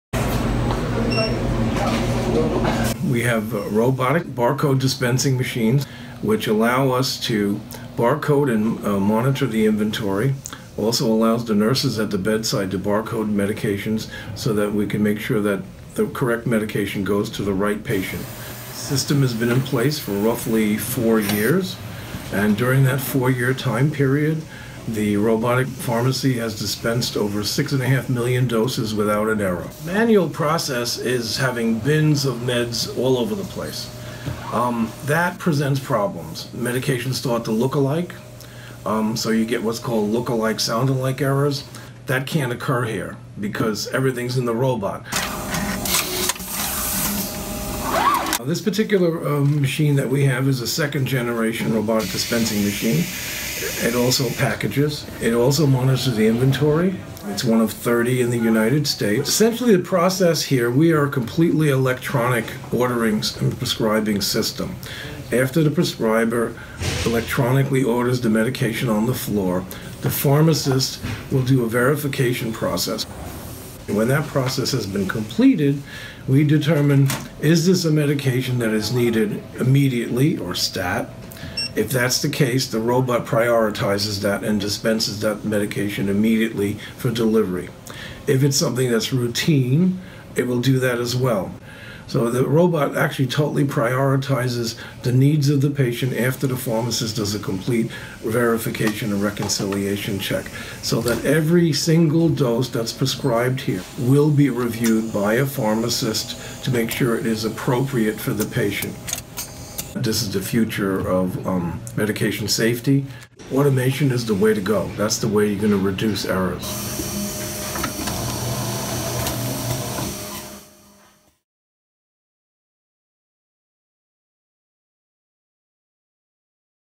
Inside NewYork-Presbyterian Queens' Robotic Pharmacy
Take a look inside NewYork-Presbyterian Queens' state-of-the-art robotic pharmacy and see how our automated system organizes, packages and monitors medications.

This spring, we also implemented a new, integrated solution and time-saving labeling system, allowing our anesthesiologists to add an extra level of safety with medication information.

NYP Queens is committed to being at the forefront of medication safety and patient care.